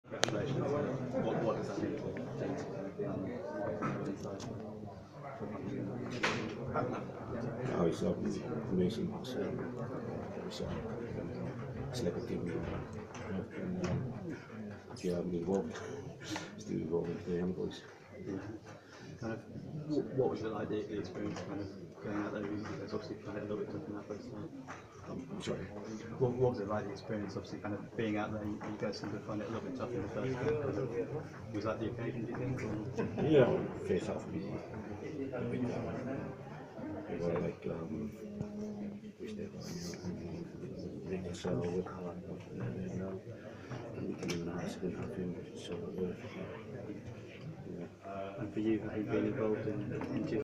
Taufa´ao Filise Challenge Cup 2018 Bibao
Final Challenge Cup

Cardiff Blues 31-30 Gloucester Rugby

Fecha: Viernes 11 mayo 2018

Estadio de San Mamés (Bilbao,España)